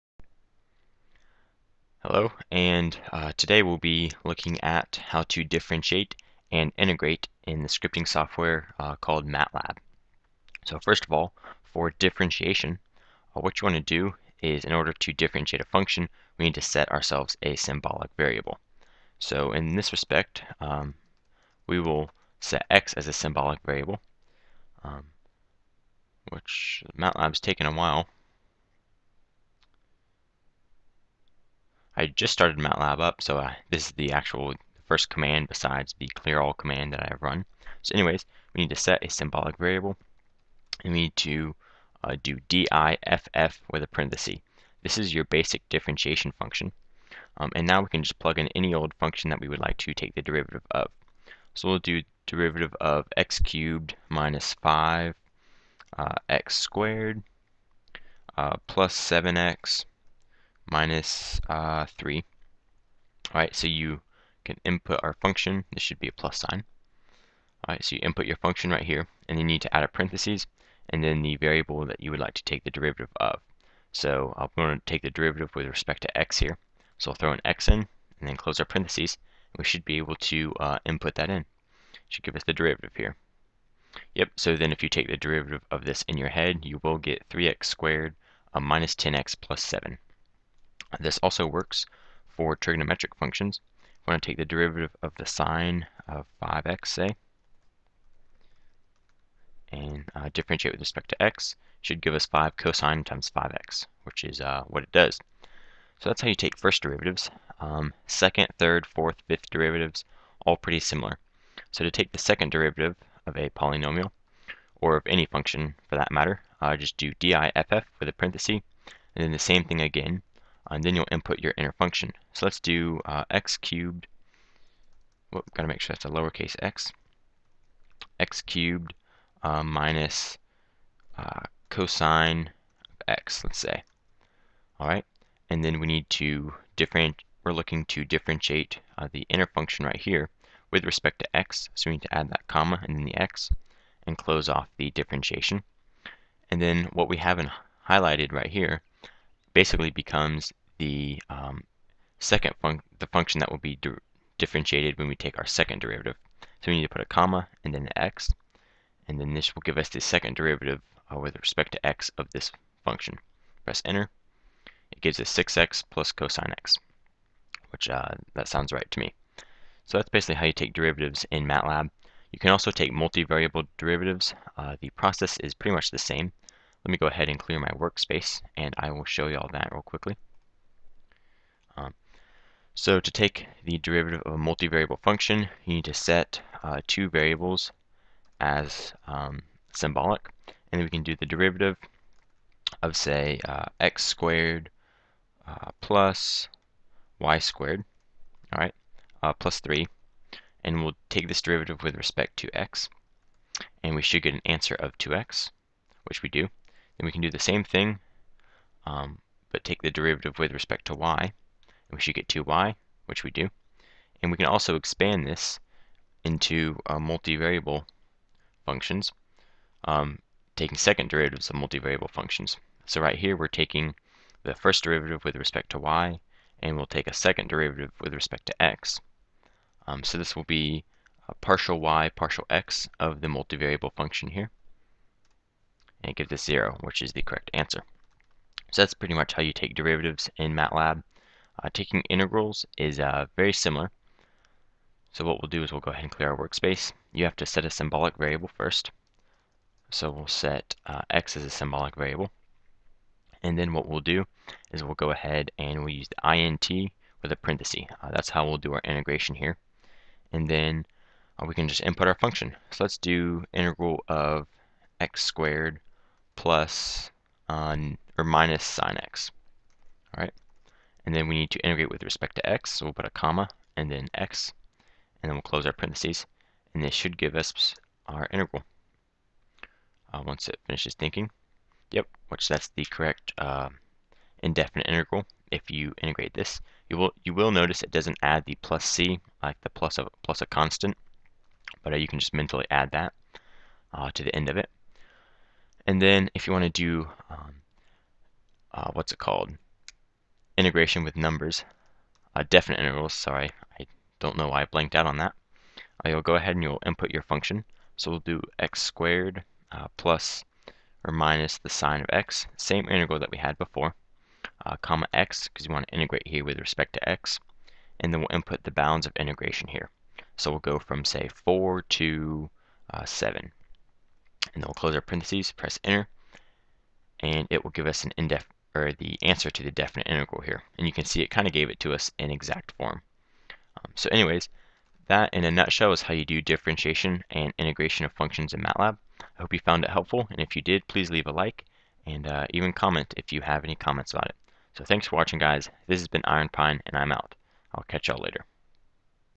Differentiation and Integration in MATLAB
Today we take a look at how to differentiate and integrate in MATLAB from a purely mathematical point of view.